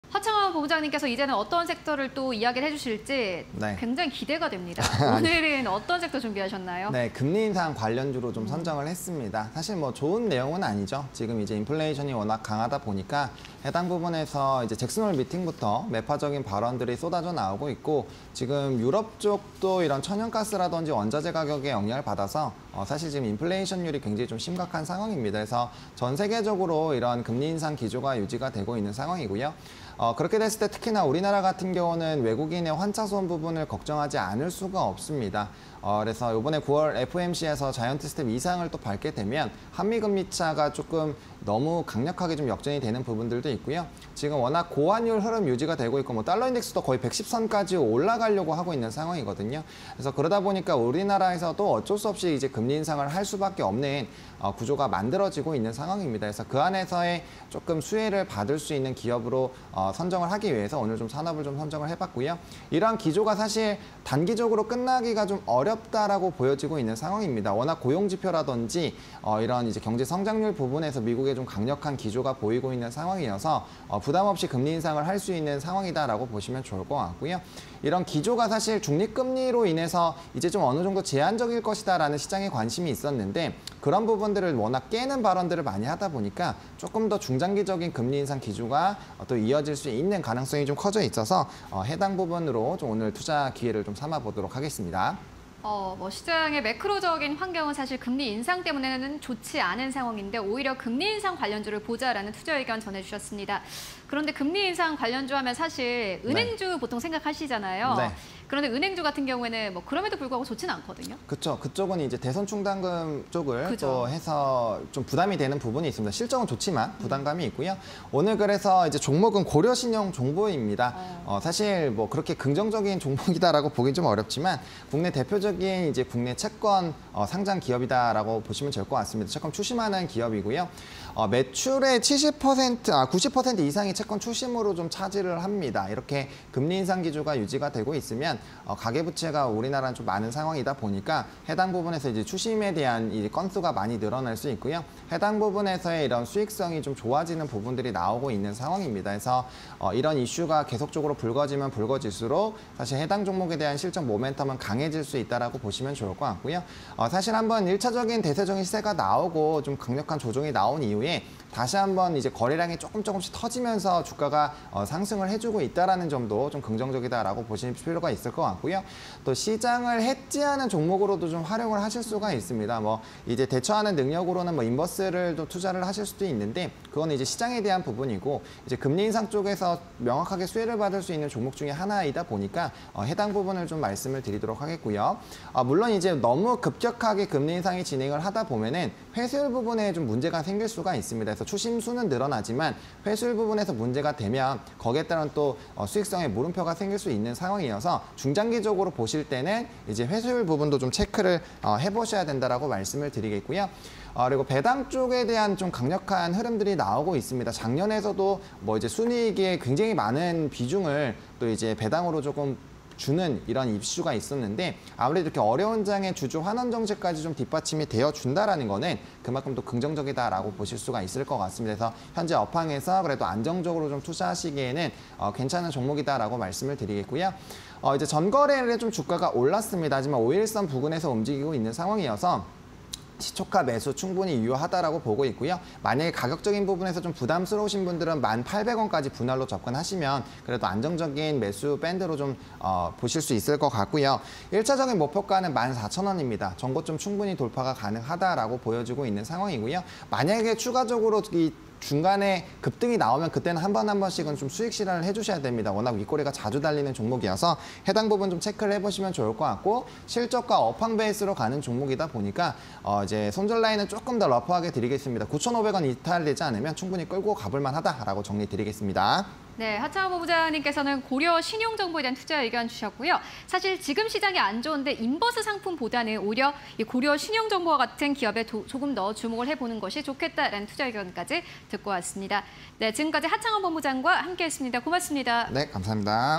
고려신용정보(049720)... 금리 인상 관련주_시초가에 이 종목 (20220902)
금리 인상 관련주
- 잭슨홀 미팅서 '금리 인상 지속' 기조 확인
- 금리 인상시 수익성 개선 섹터 관심 필요
- 기후·원자재 가격 변동 따른 인플레 부담 여전
- 한·미 금리역전… 한은 금리 인상 가능성↑

고려신용정보(049720)
- 국내 대표 채권 추심 기업
- 시장 점유율 17.2%의 유일한 상장사
- '채권 추심' 전체 매출의 90% 차지
- 회수한 채무에 대한 수수료가 주요 수익원
- 경기 둔화 등 부실채권 증가… 호실적 전망
- 금리 인상 기조 지속 따른 수혜 예상
- 중간배당 등 주주환원정책 시행
- 분기배당 1주당 160원… 배당금 매년 증가
- 순이익 감소에도 배당금 확대